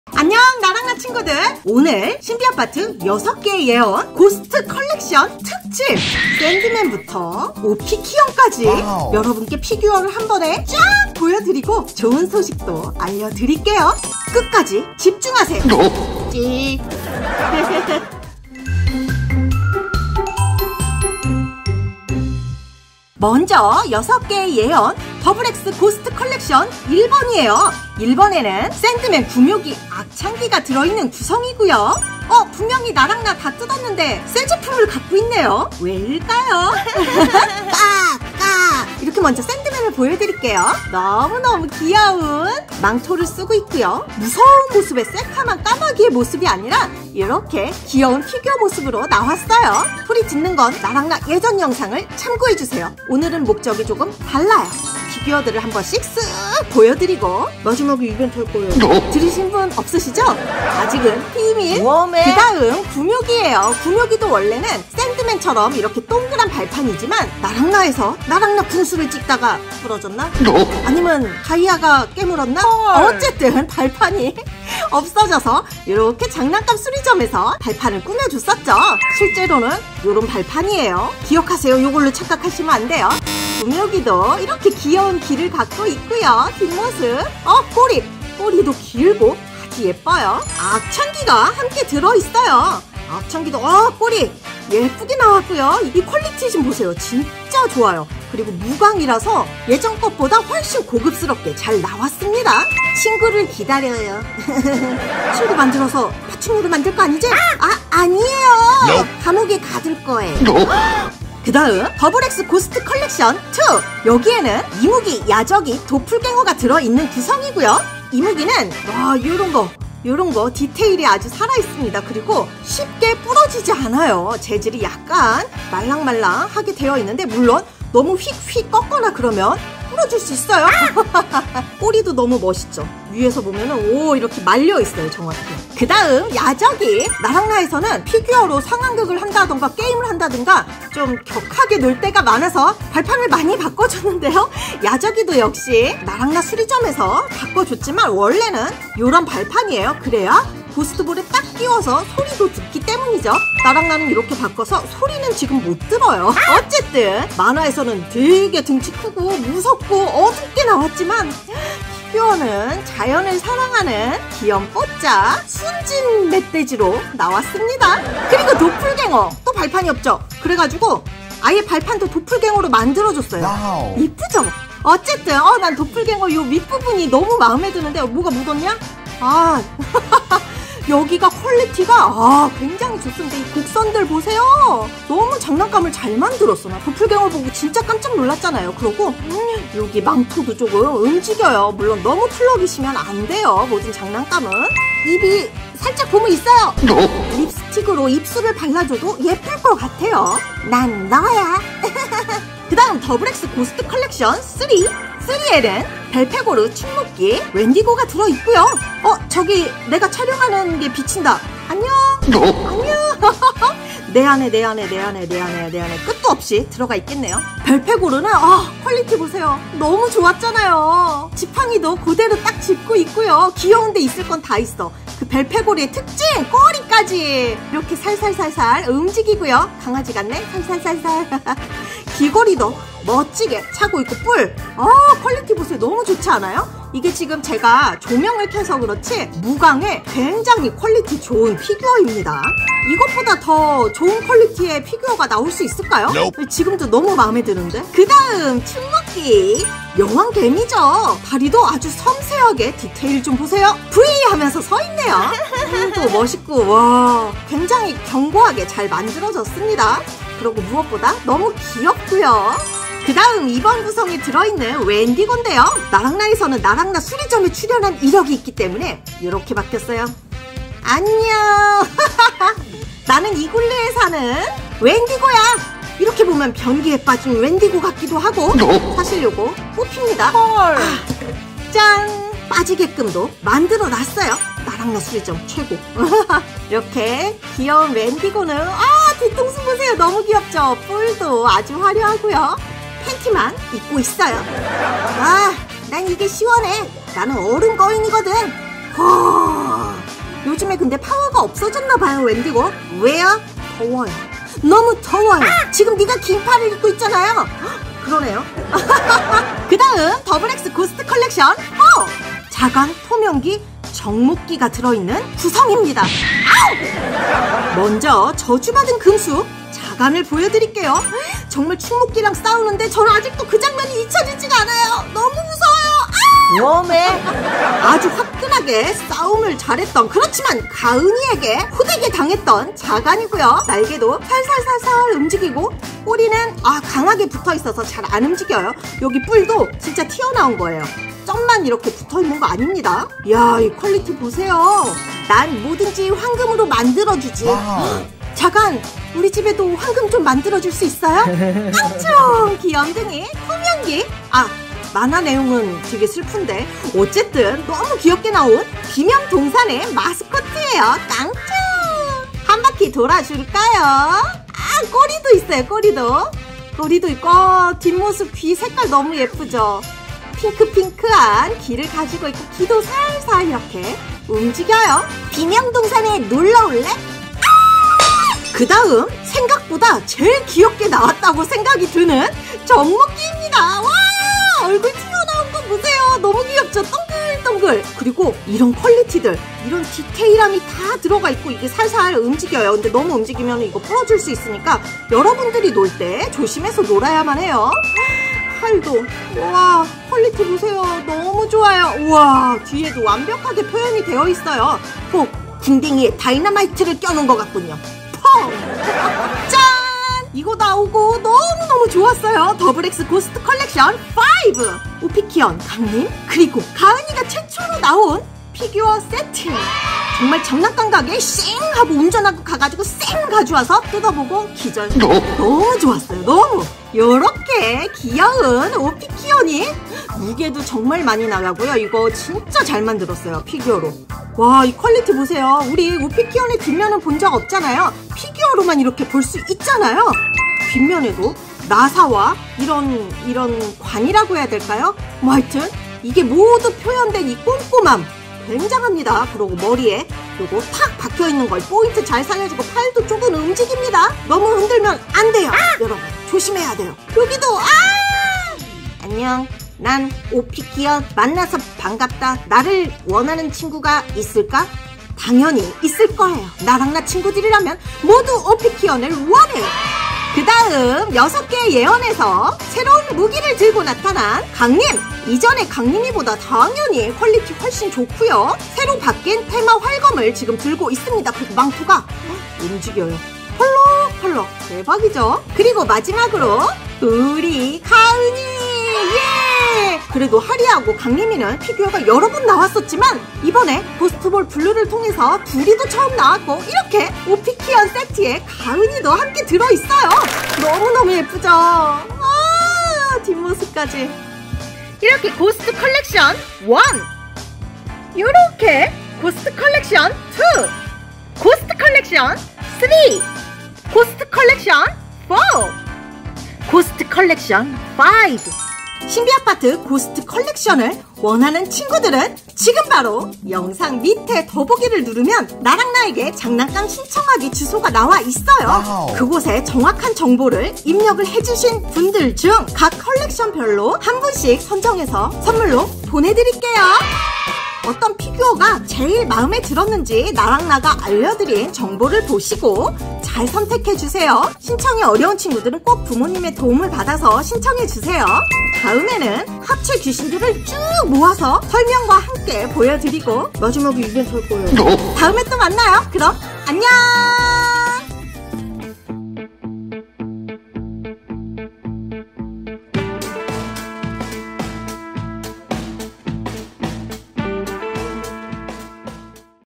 몽땅~~ 신비아파트3 👻 고스트 피규어 자세히 보기!! | 나에게 맞는 피규어는?
신비아파트 시즌3 6개의 예언 피규어들을 모두 모아서,
피규어 설명과 함께 자세히 보여드려요.
각 특징들을 보시고, 원하시는 피규어를 마음속에 저장한다음,
아래의 링크에서 신청해주세요!! 😍😍😍

* 고스트 컬렉션 세트를 원하는 친구들은 아래의 링크에서 신청서를 작성해주세요.

* 신청기간 : 09월 03일 목요일 ~ 09월 06일 일요일
* 결과발표 : 나랑나 채널 커뮤니티에 09월 08일 화요일(예정)

* 제품정보
   제품명 : 신비아파트3 더블X 고스트 컬렉션1, 2, 3, 4, 5
   판매원 : 오로라월드((주)
* 본 영상에서 제공해드리는 제품은 나랑나 자체구입 + 일부 오로라월드에서 지원해주셨습니다. 감사합니다~💕